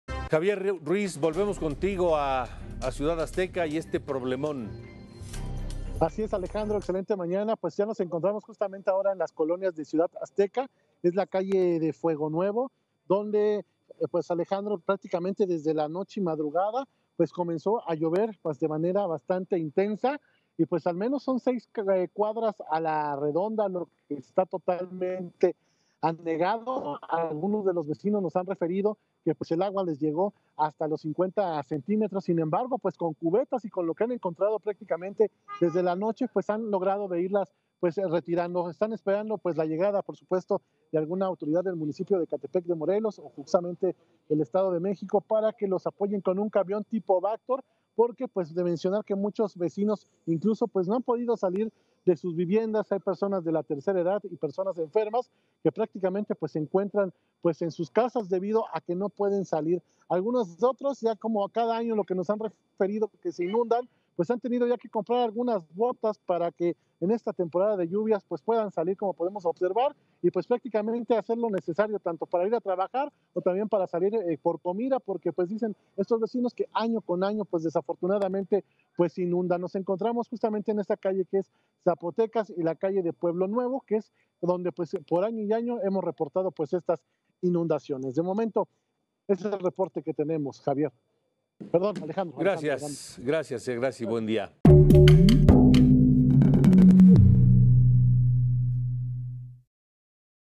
¡Igual que en Chalco! Fuertes lluvias causan severas inundaciones en Ecatepec
Durante la mañana del 29 de agosto las calles de Ecatepec sufrieron graves inundaciones por las fuertes lluvias a lo largo de la madrugada. La abundante presencia de agua complicó la circulación vial y dejó varados a numerosos automovilistas, principalmente en avenida Central y alrededor de la zona de Ciudad Azteca.

Aunado a esto, el transporte del Mexibús también se ha visto afectado, puesto que no ha podido circular debido al tránsito vial y la circulación de agua en las calles, así como los obstáculos encontrados en las vías por las intensas precipitaciones.